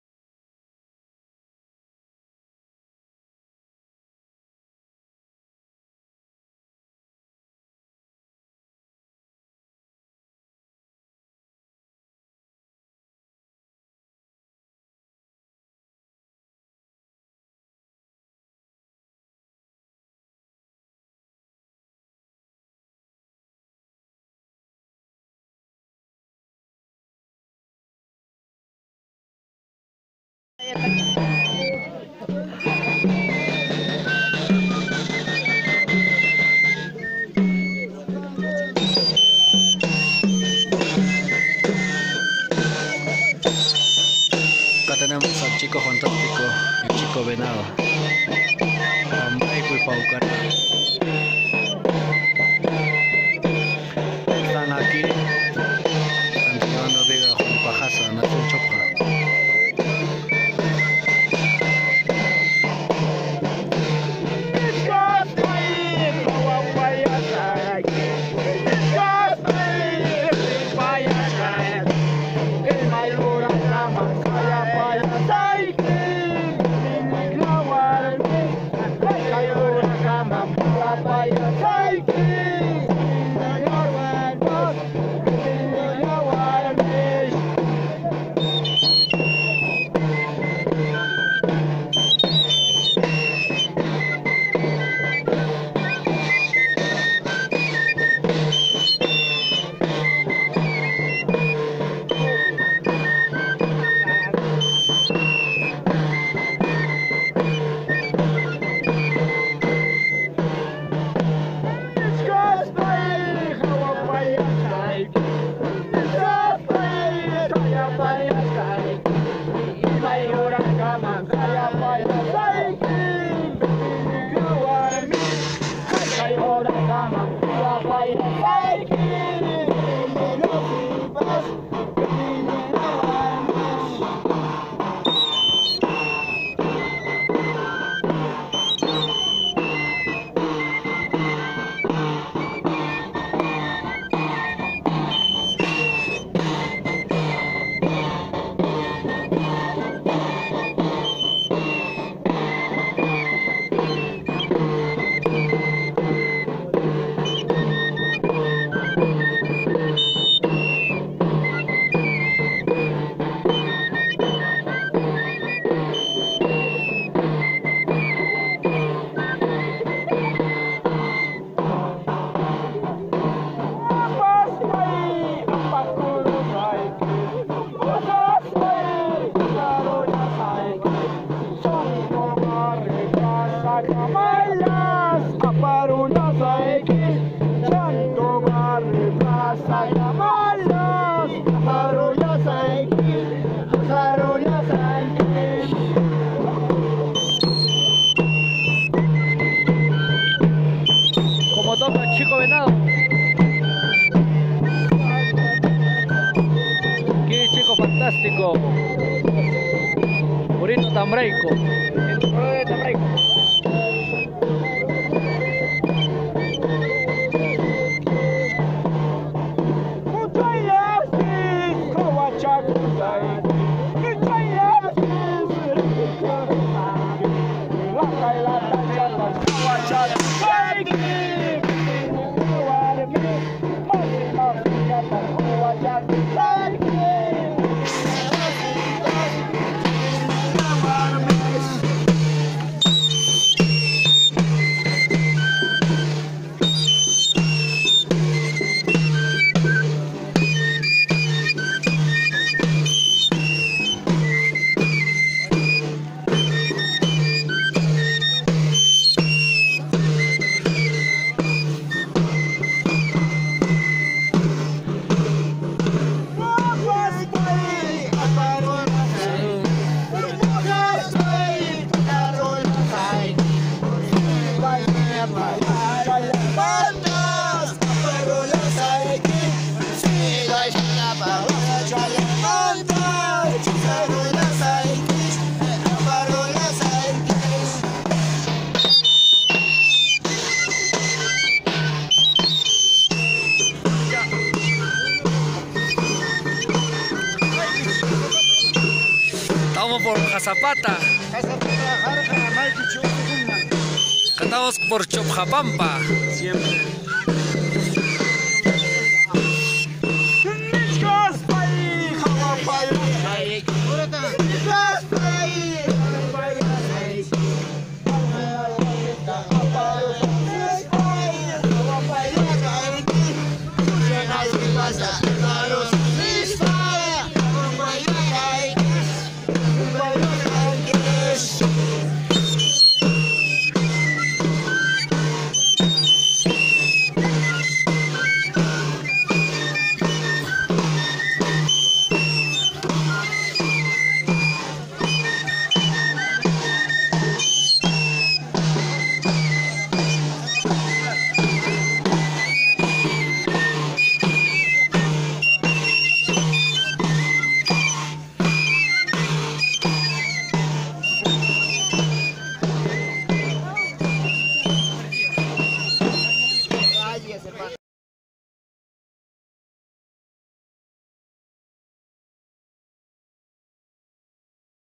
Músicas del Perú: Pinkullo en la fiesta-faena del Viga Wantuy.
En los Tambos (lugares donde descansa la viga) se
siguen entonando diversas melodias de la Tonada
"PASO" (tonada para trasladar de la viga). Aqui si
observa a los Maestros Pikulleros Fredy de Tambraico
popular "Chico Fantastico" y Sebastian de Paucara,
popular "Chico Venado", realizando TAKYS y los
aficionados venidos de Chopccapampa, Ccasapata,
Sotopampa entre otros se incorporan para cantarle a
la Alisuna Warmi (Mujer de Aliso) nombre dado al
Arbol.
Jonathan Berrocal Casimir " Qory Purikuq"